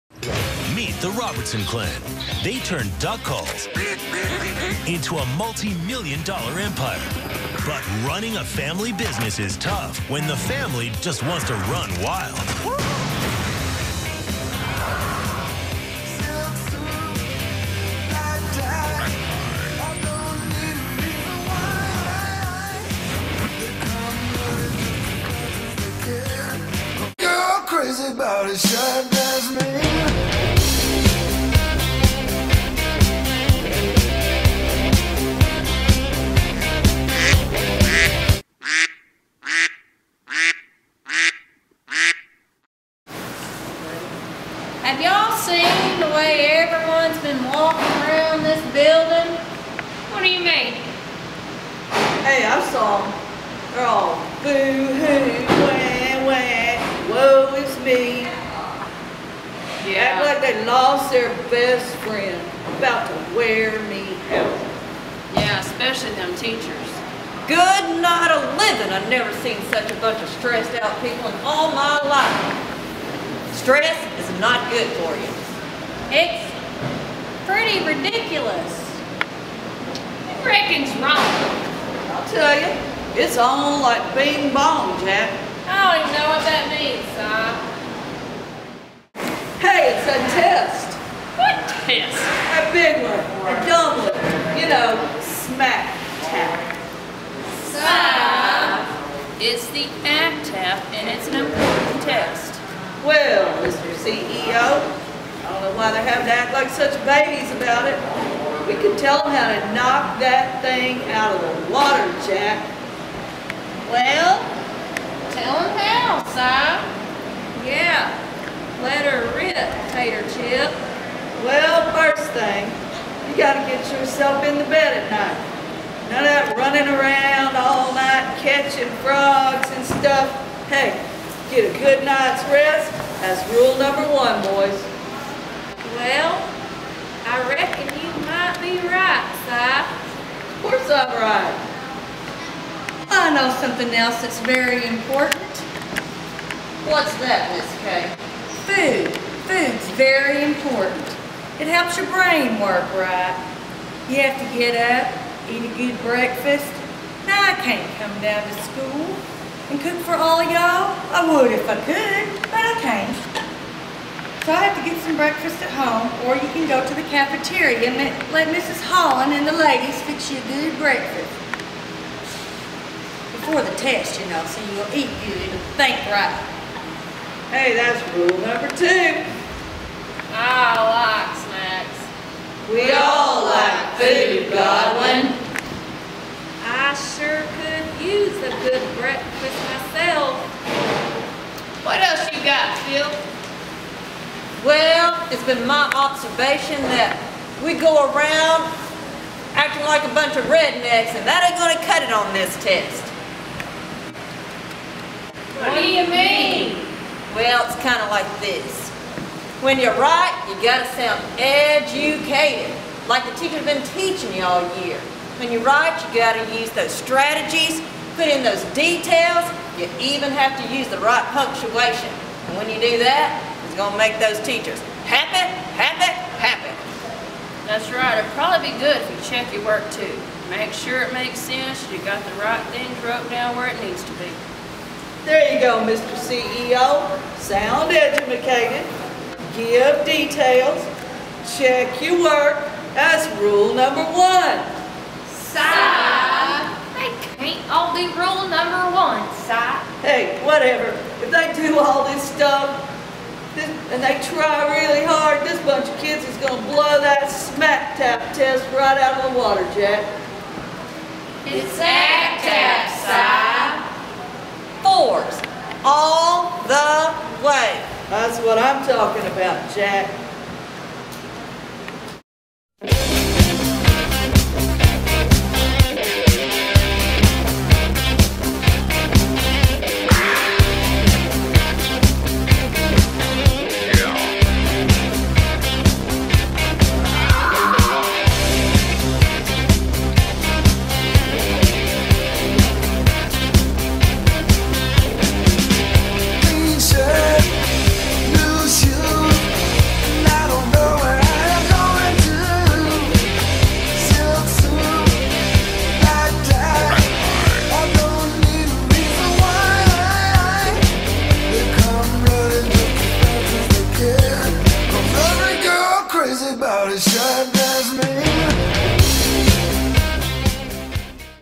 Duck Dynasty Tips for the ACTAAP ft. Pyron Elementary
Pyron Elementary's 3rd Grade teachers are members of the Robertson Clan from West Monroe, LA.  Listen while they give good tips when taking the ACTAAP!